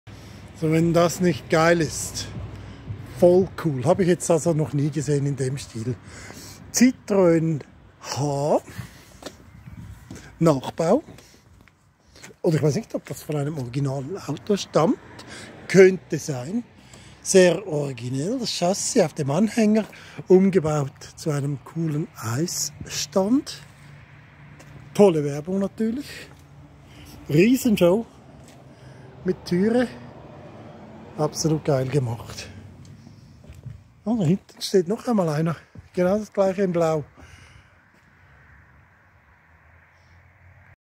Citroën Typ H Icecream Anhänger
Das ist nur ein bisschen ein Citroën Typ H! Originell er ist zu einem Icecream Anhänger umgewandelt worden. Eventuell auch komplett nachgebaut!